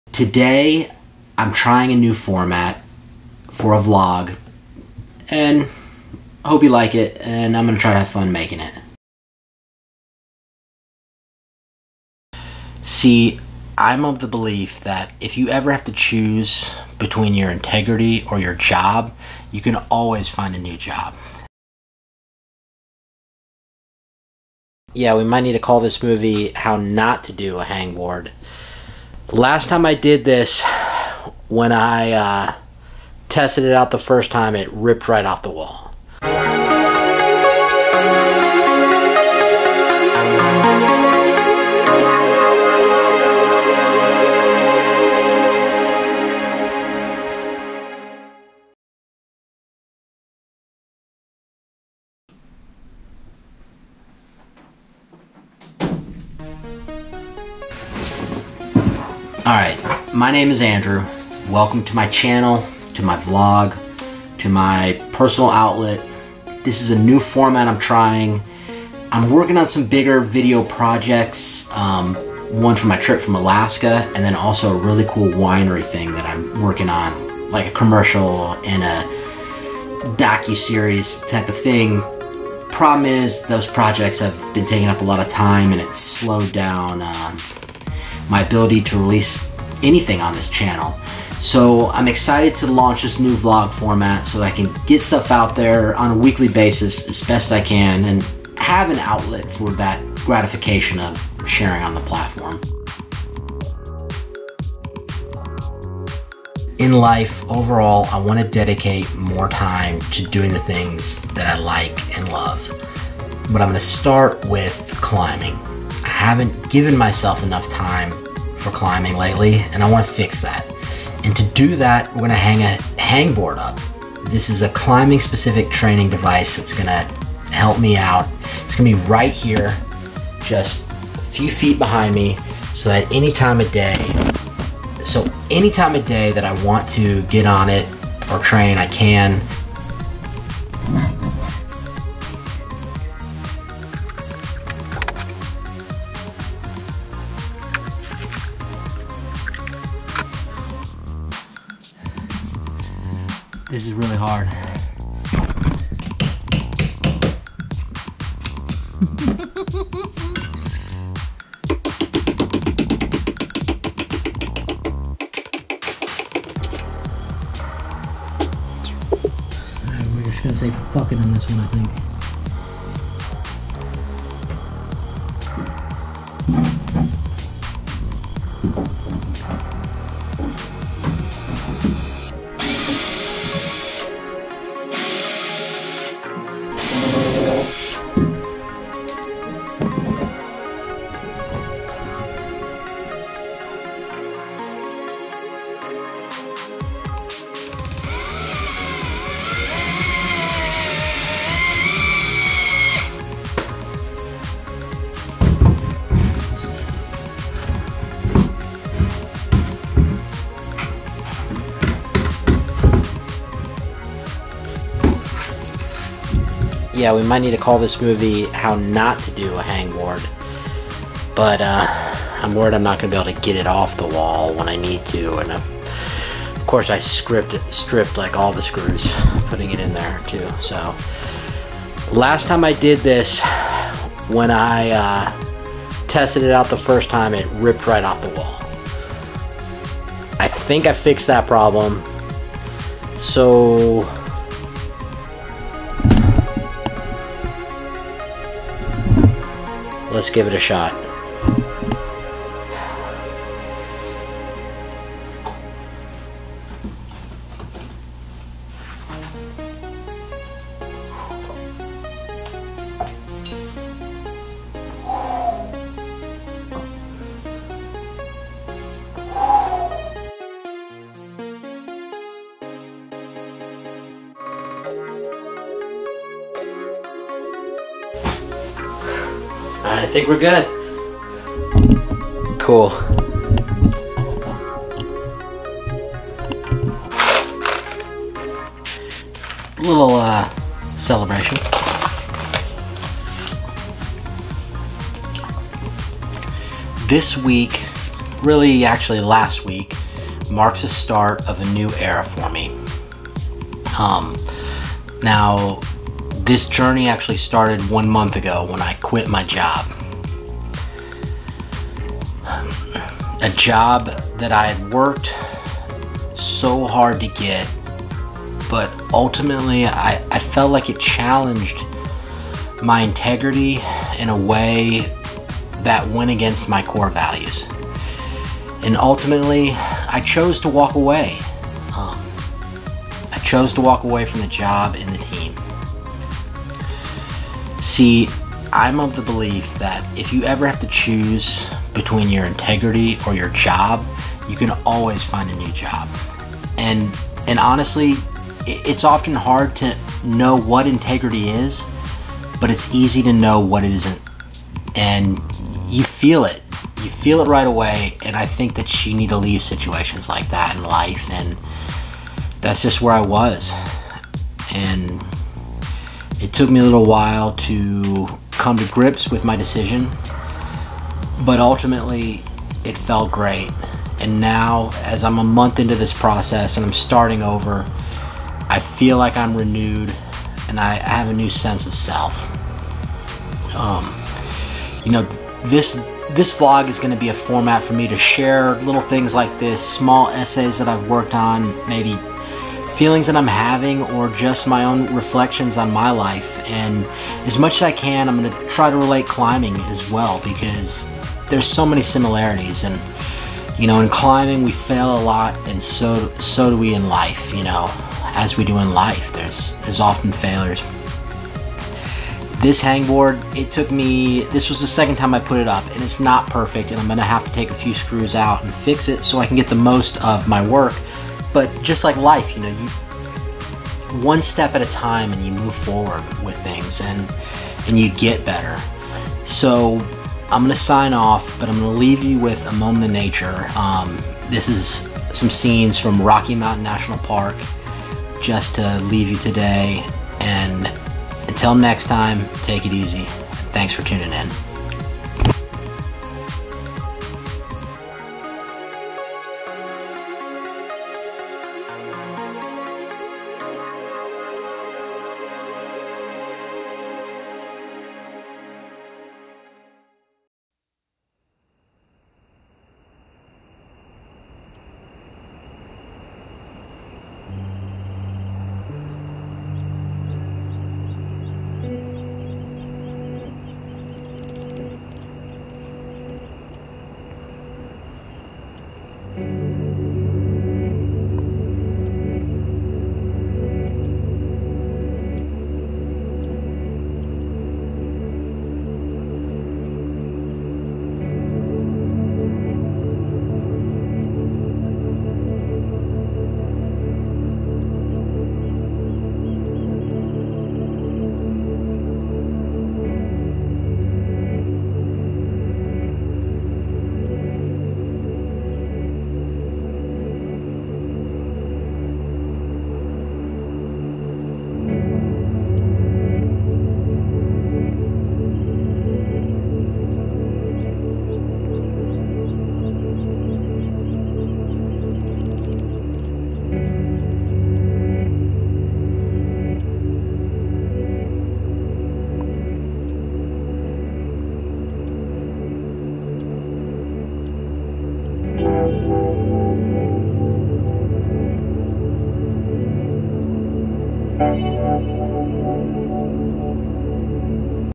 New Beginnings, Vlog #1 - Installing a hang board and discussion on personal integrity.
This new vlog series marks a new beginning for me and it starts with the hardest decision of my life. Also in this episode I set up a hangboard in my office to help motivate me toward my goal of climbing more.